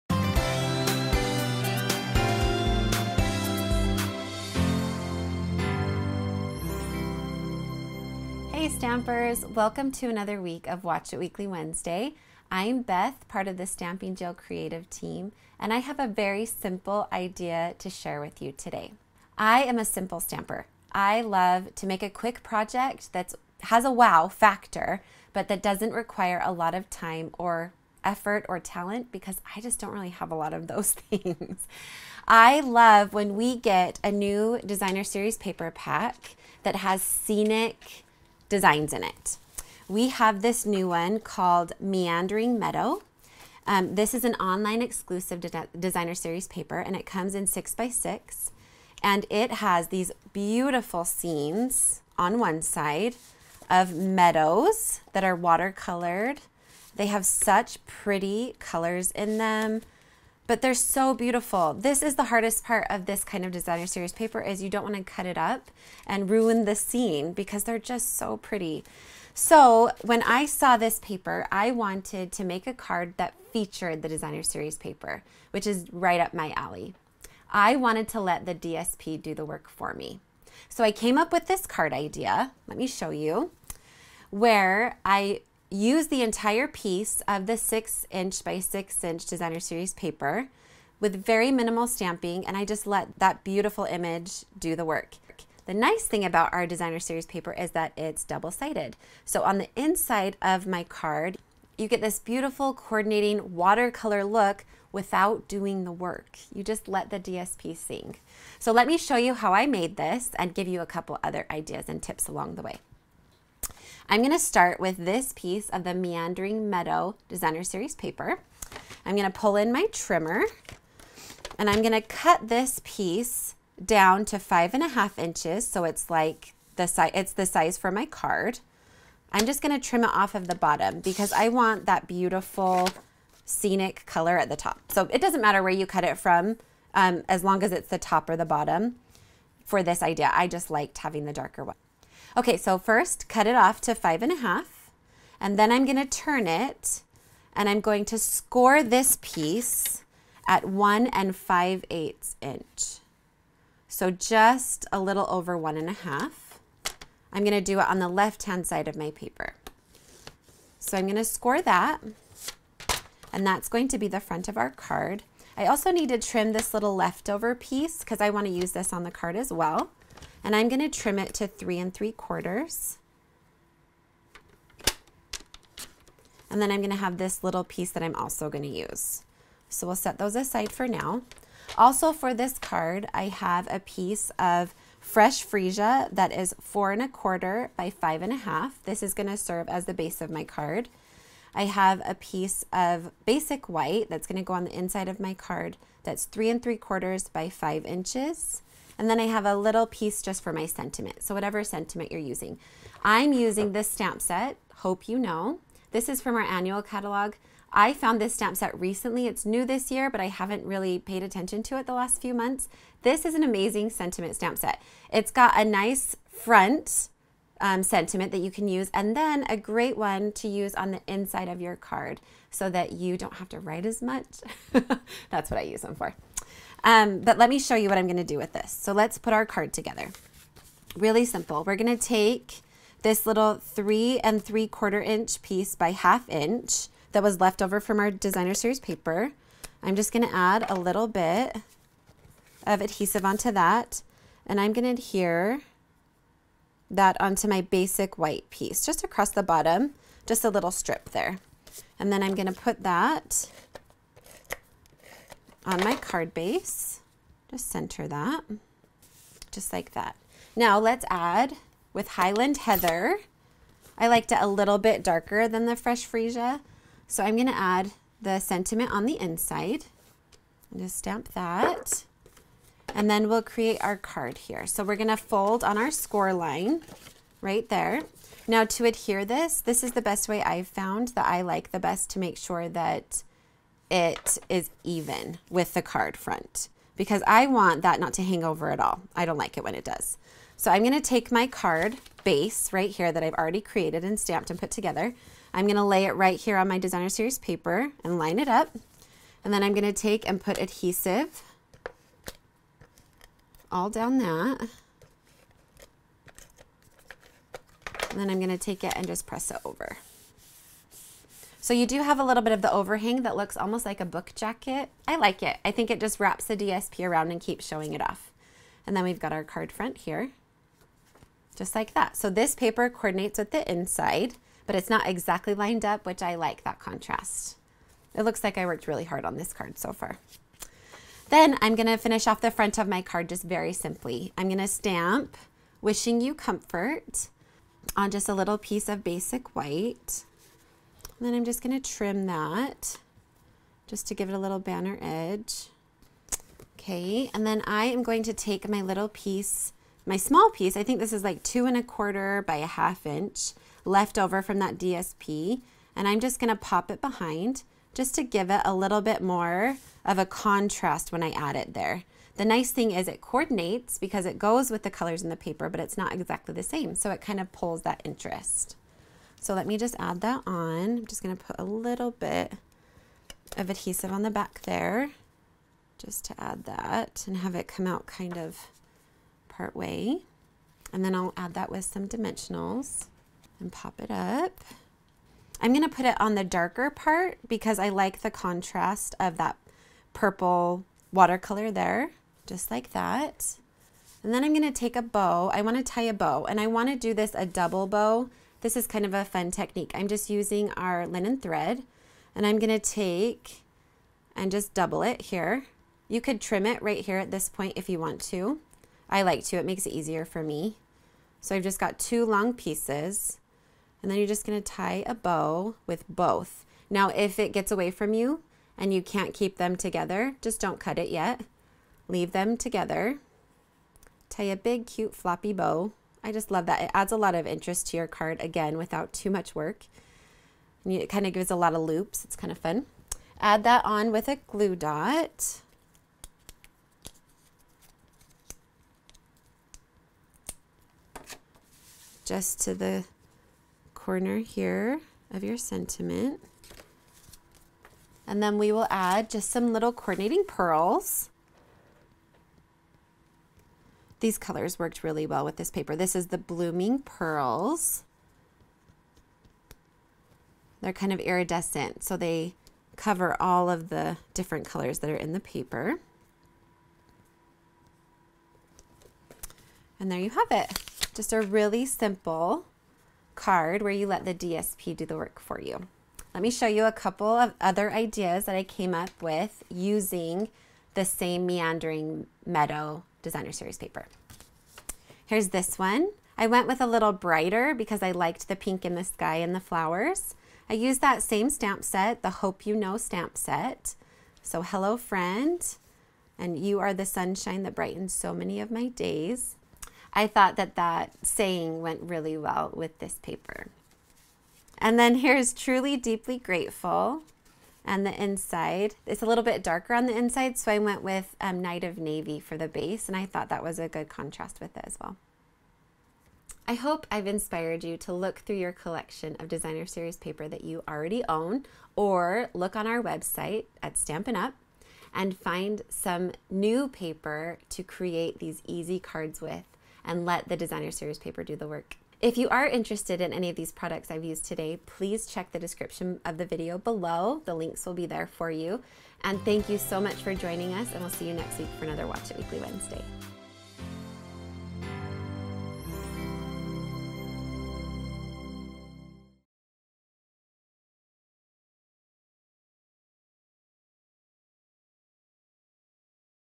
Stamping Jill - Let The DSP Do The Work!
Stampin' Up! Demonstrator Beth Olsen demonstrates the beautiful Designer Series Paper (dsp) and how easy you can make a beautiful card using one sheet of 6x6 designer series paper. (dsp) Let the dsp do the work and make you look like a creative super hero, Check it out!

Meandering Meadows 6" X 6" (15.2 X 15.2 Cm) Designer Series Paper [162735]